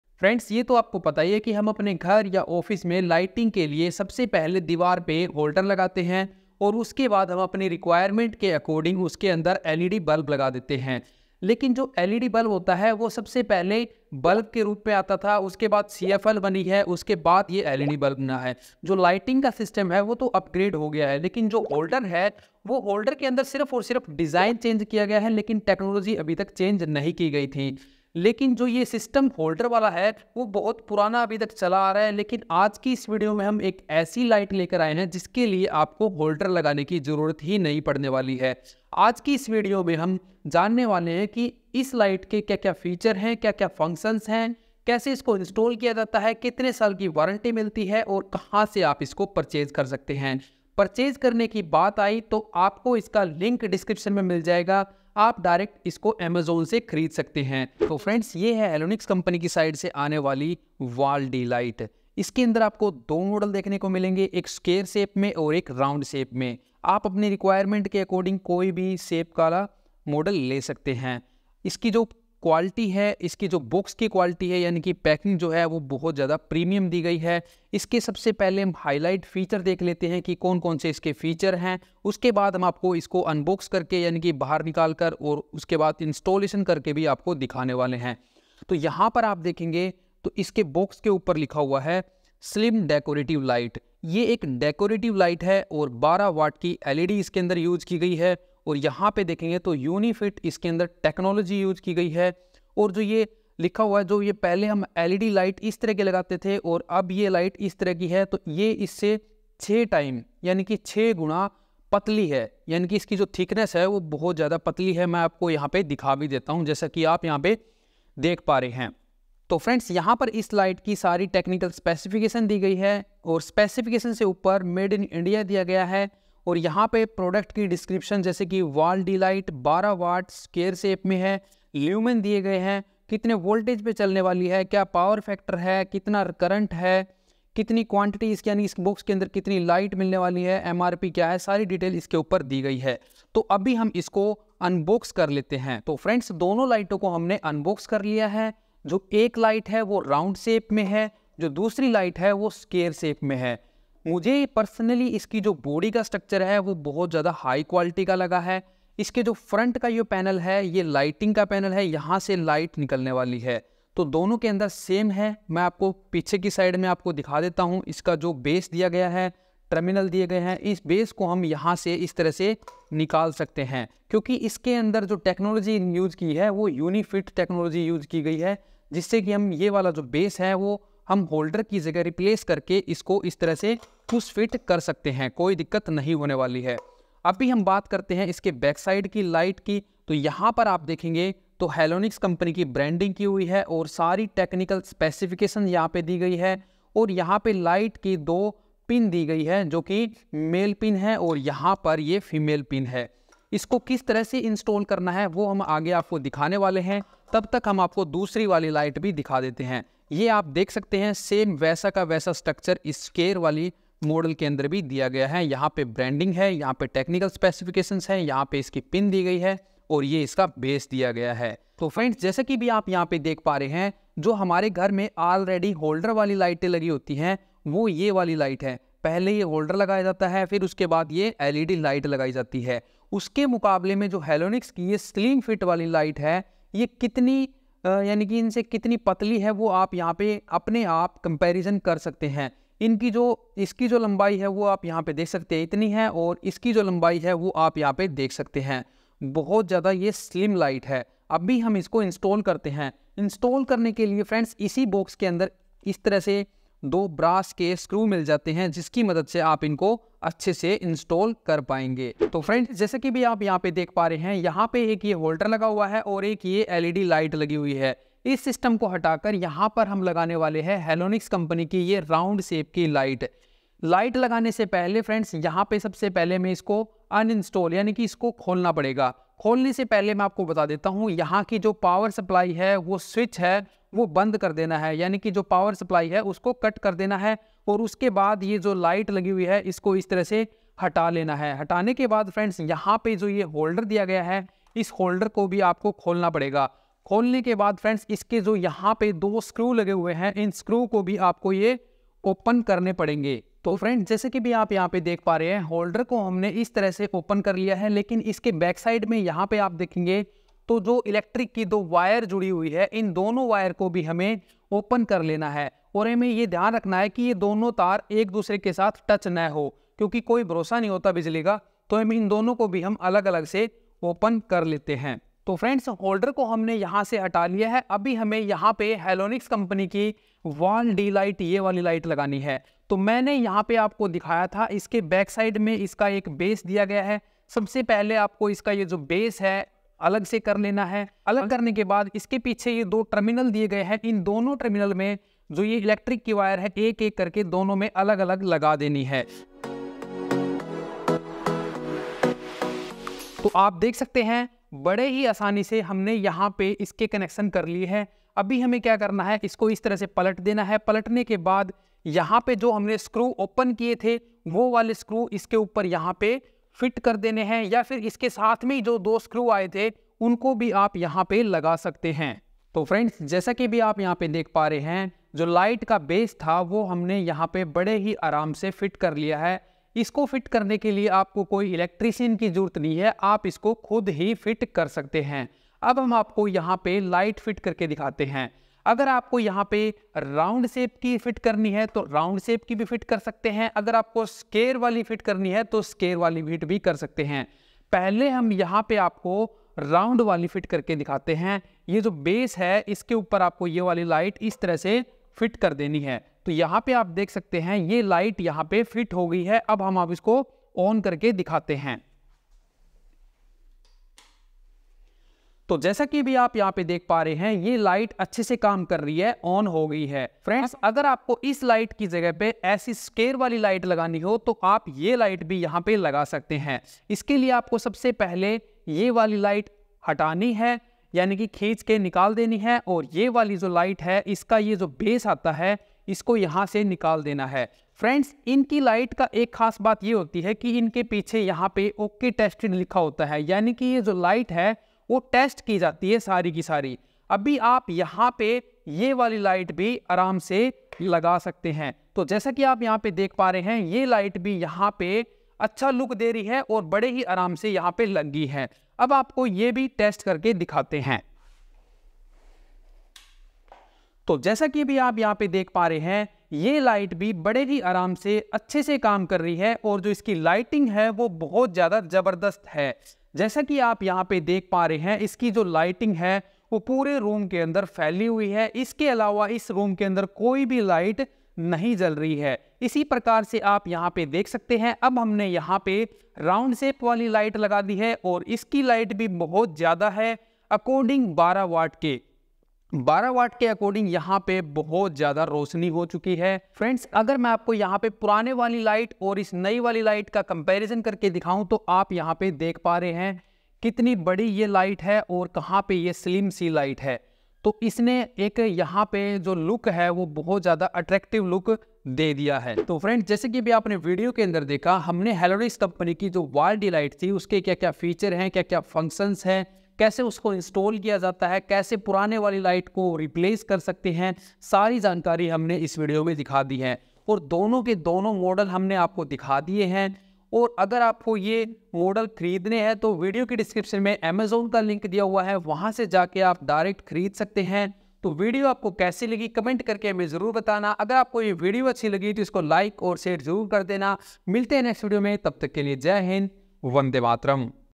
Halonix Prime 12 Watt Wall DE-Light Full Details and Installation | Best Light For Home and Office
friends is video me aap dekhnge ghar or office ke liye ek latest wall light joki ek holder free light hai or bahut jyada slim hai yh light Halonix company ki side se aati hai

Light Khridne ka link